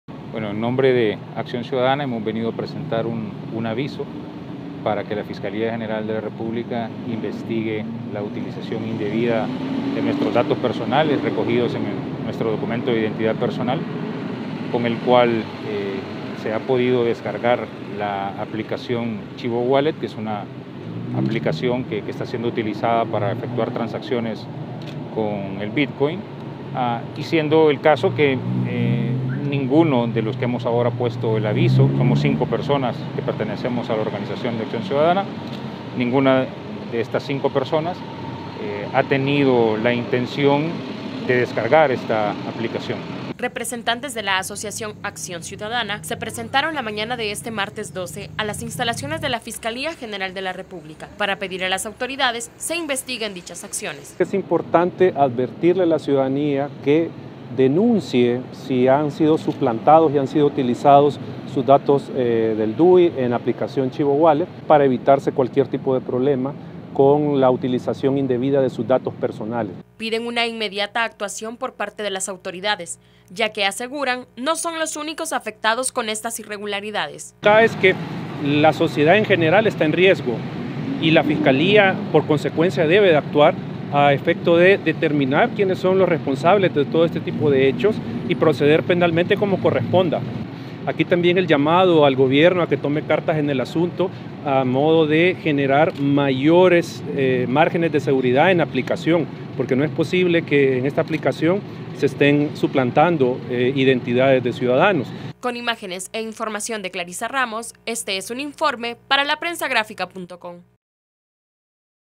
Acción Ciudadana presenta aviso para que investiguen presuntos delitos de uso de datos personales
Los titulares de la asociación externaron su preocupación ante el uso que se ha hecho de sus datos sin tener conocimiento de los fines que estas acciones podrían tener.

¡Visítenos!